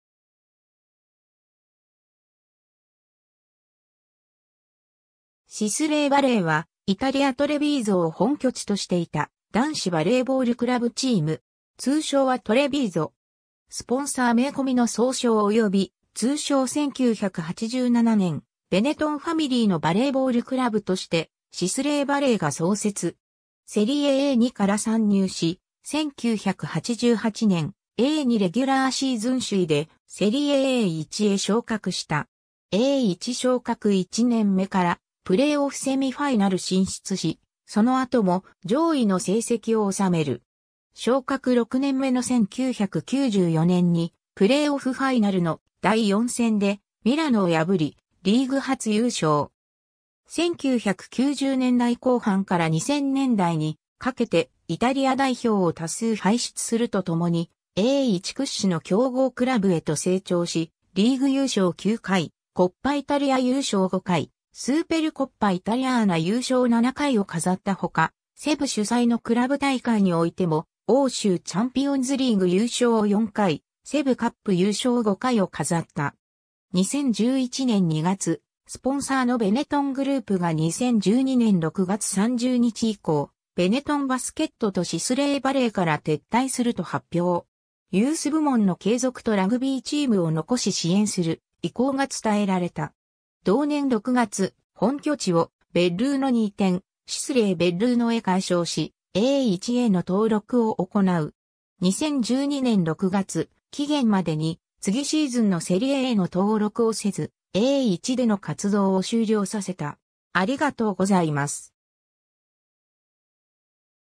シスレー・バレー
シスレー・バレー（Sisley Volley）は、イタリア・トレヴィーゾを本拠地としていた男子バレーボールクラブチーム。
通称はトレヴィーゾ。
 スポンサー名込みの総称および通称 1987年、ベネトンファミリー（famiglia Benetton）のバレーボールクラブとして、シスレー・バレーが創設。
セリエA2から参入し、1988年A2レギュラーシーズン首位でセリエA1へ昇格した。
 A1昇格1年目からプレーオフ・セミファイナル進出し、その後も上位の成績を収める。
昇格6年目の1994年にプレーオフ・ファイナルの第4戦でミラノを破り、リーグ初優勝。
1990年代後半から2000年代にかけてイタリア代表を多数輩出するとともに、A1屈指の強豪クラブへと成長し、リーグ優勝9回、コッパ・イタリア優勝5回、スーペルコッパ・イタリアーナ優勝7回を飾ったほか、CEV主催のクラブ大会においても欧州チャンピオンズリーグ優勝4回、CEVカップ優勝5回を飾った。
 2011年2月、スポンサーのベネトングループが2012年6月30日以降、ベネトン・バスケットとシスレー・バレーから撤退すると発表。
ユース部門の継続とラグビーチームを残し支援する意向が伝えられた。
同年6月、本拠地をベッルーノに移転、シスレー・ベッルーノへ改称し、A1への登録を行う。
 2012年6月、期限までに次シーズンのセリエAの登録をせず、A1での活動を終了させた。